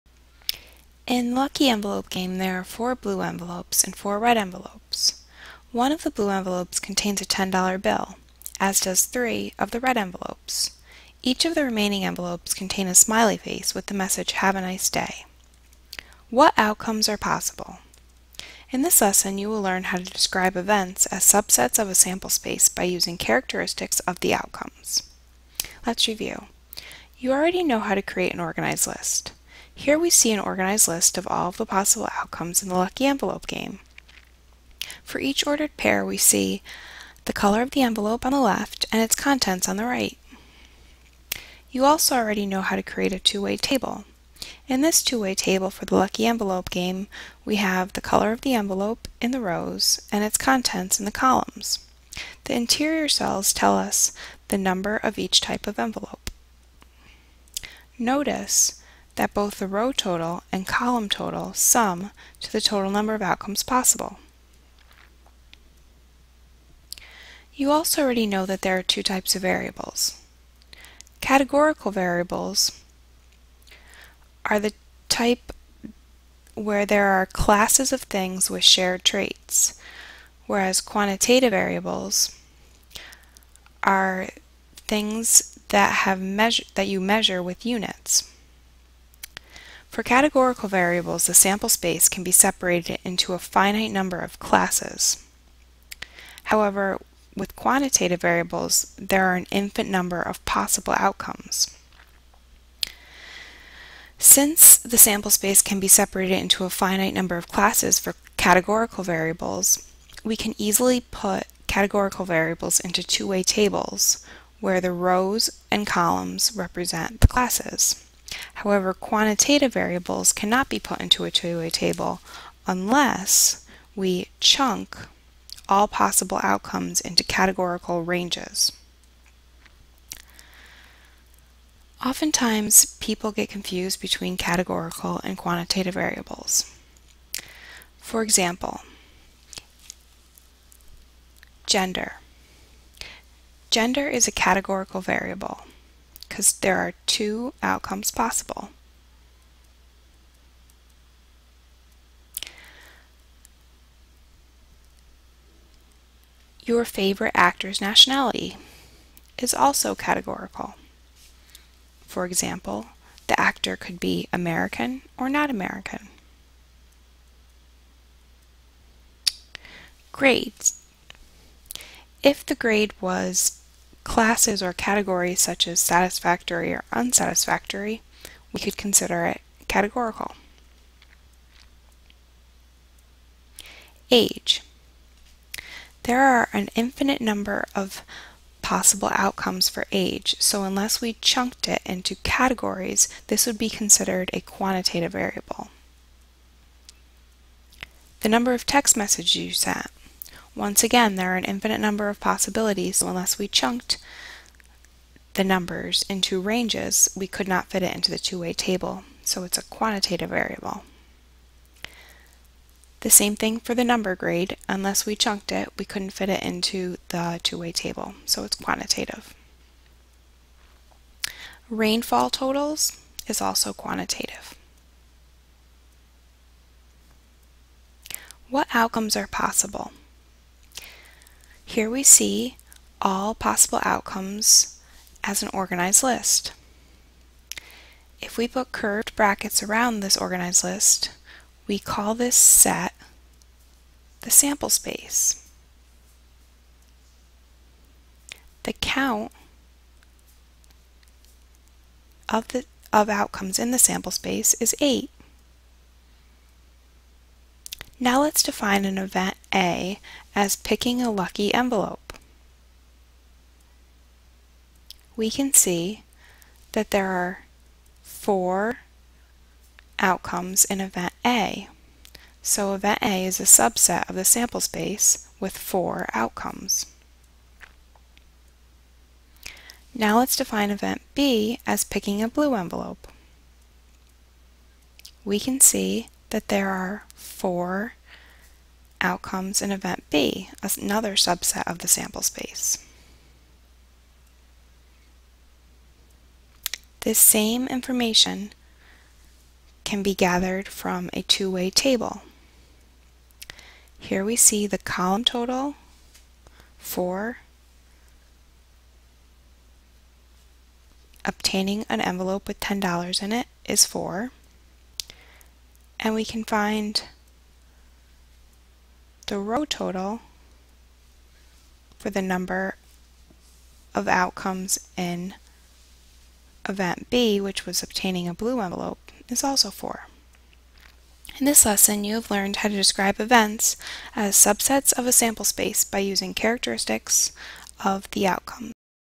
Describe events as subsets of a sample space
In this lesson, you will learn how to describe events as subsets of a sample space by using characteristics of the outcomes.

ADDITIONAL MATERIALS

STANDARDS

CCSS.HSS-CP.A.1 Describe events as subsets of a sample space (the set of outcomes) using characteristics (or categories) of the outcomes, or as unions, intersections, or complements of other events (“or,” “and,” “not”).

IN.FM.S.2 Perform operations on sets (union, intersection, complement, cross product) and illustrate using Venn diagrams.

IN.FM.P.8 Describe events as subsets of a sample space (the set of outcomes) using characteristics (or categories) of the outcomes, or as unions, intersections, or complements of other events.

FL.MAFS.912.S-CP.1.1 Describe events as subsets of a sample space (the set of outcomes) using characteristics (or categories) of the outcomes, or as unions, intersections, or complements of other events (“or,” “and,” “not”).